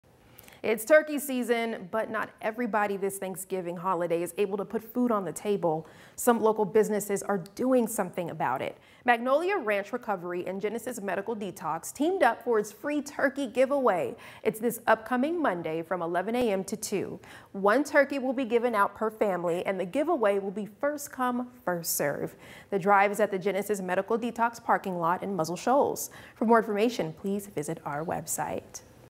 Here's your chance to win a free turkey!
Magnolia Ranch Recovery and Genesis Medical Detox team up for a free turkey giveaway